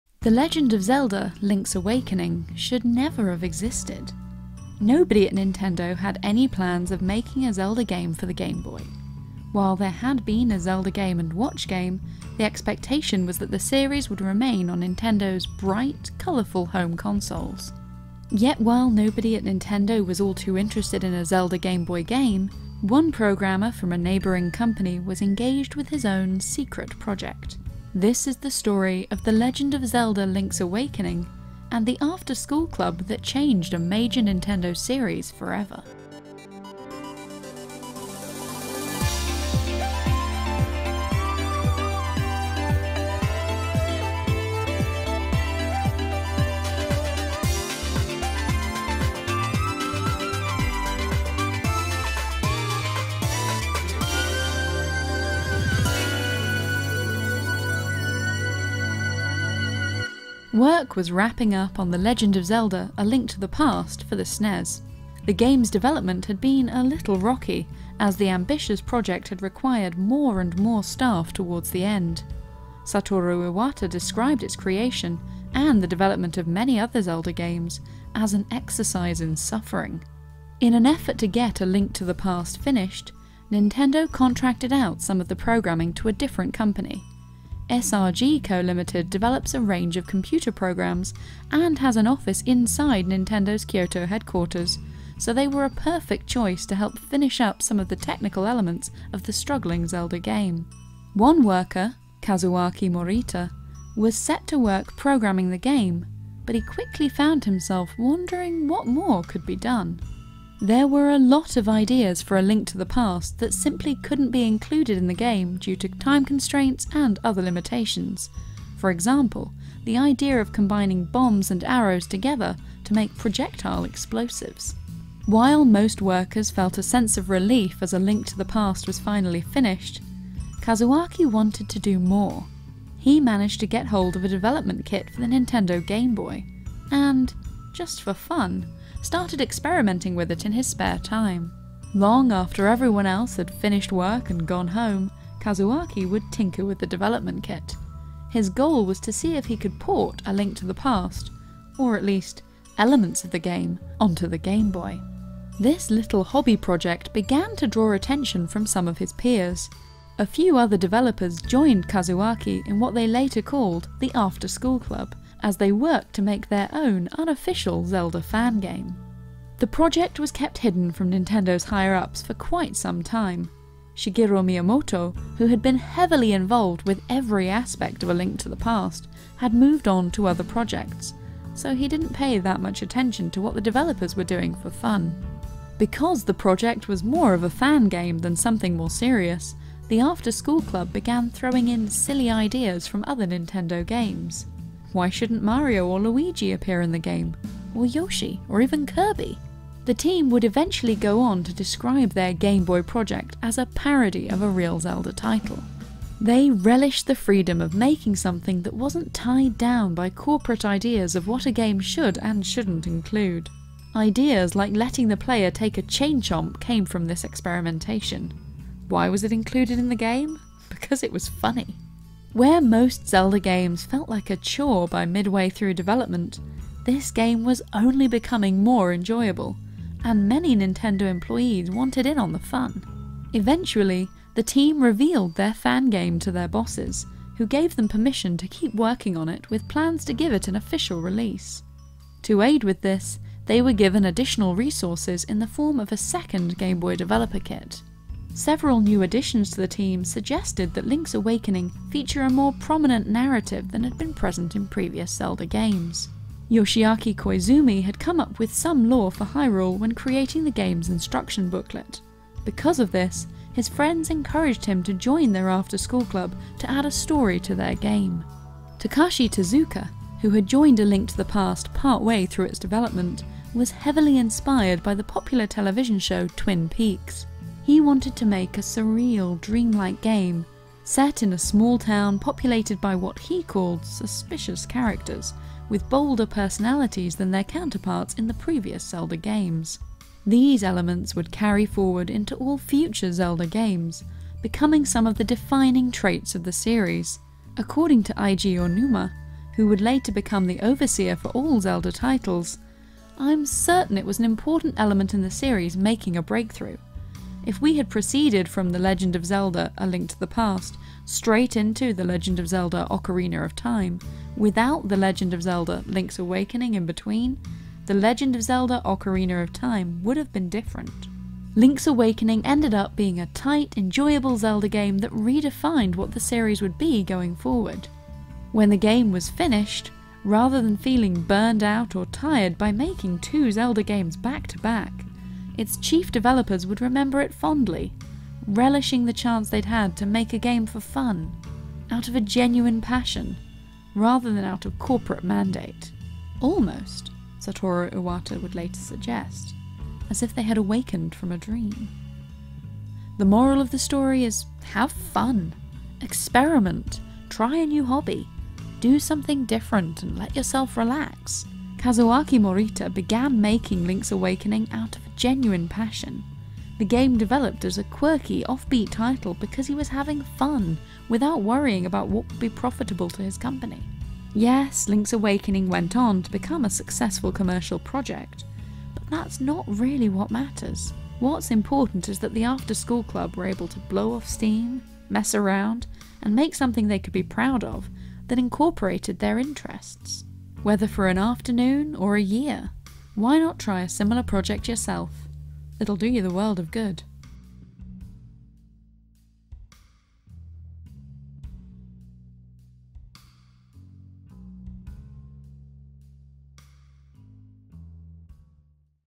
Link's Awakening Started as an Unofficial Fan Game
Did you know that Link's Awakening started life as a fan game? Its original developer, Kazuaki Morita, didn't even work for Nintendo! He worked on A Link to the Past when his programming company was contracted to help build the game, and afterwards, he helped himself to a Gameboy development kit to try porting Zelda onto the Gameboy.

Then, he and some friends from Nintendo started doing funny stuff like adding in Yoshi and Chain Chomps mostly just for fun. The result is one of the most irreverant and meta games in all of Zelda canon.

No, by the way, we haven't played the Switch remake of Link's Awakening just yet. It's on its way in the post alongside a Switch Lite. It's cool, though, we know the twist at the end: Koholint Island was really Subcon all along!